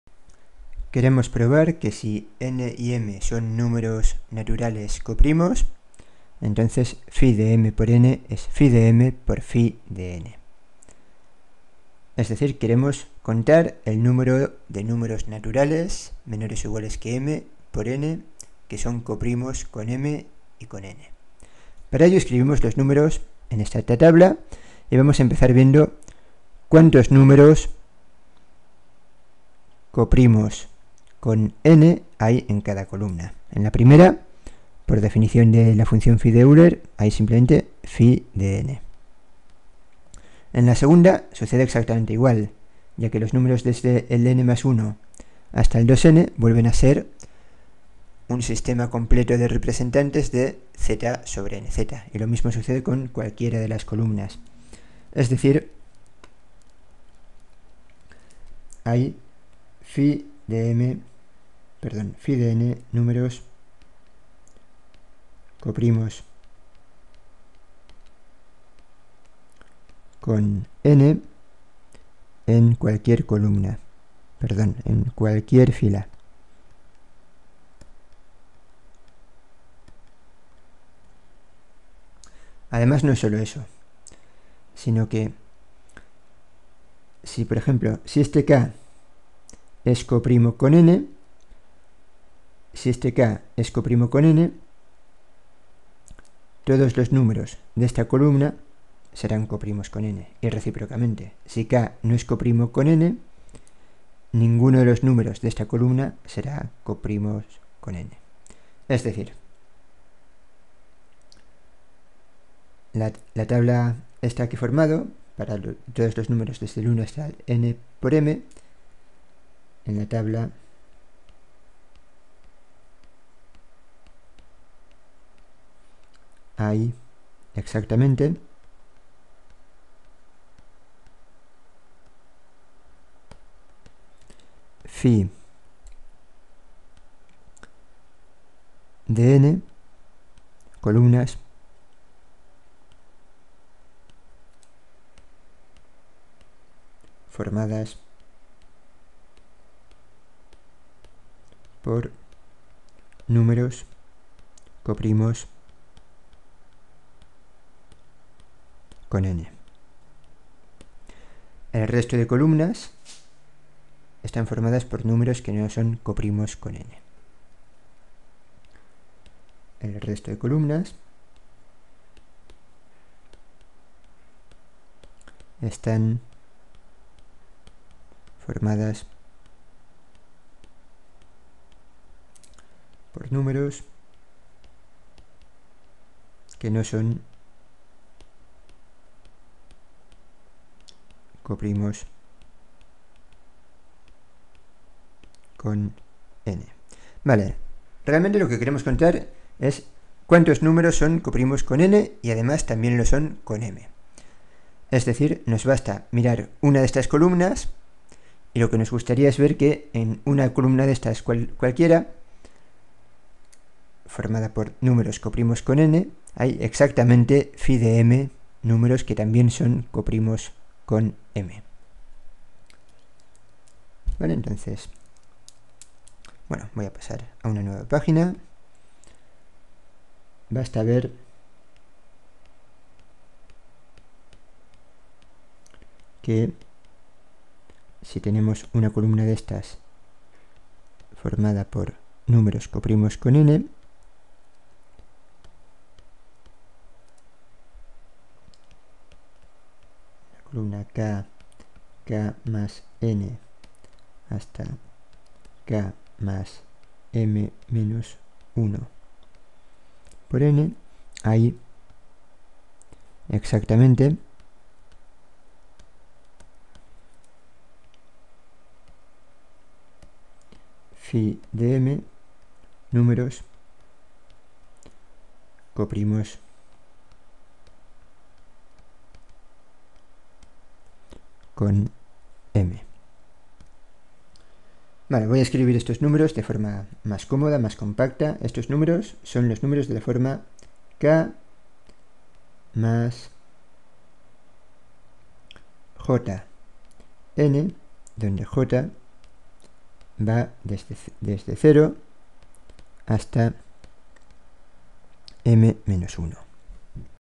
La propiedad multiplicativa de la función de Euler
Demostramos que la función de Euler es multiplicativa.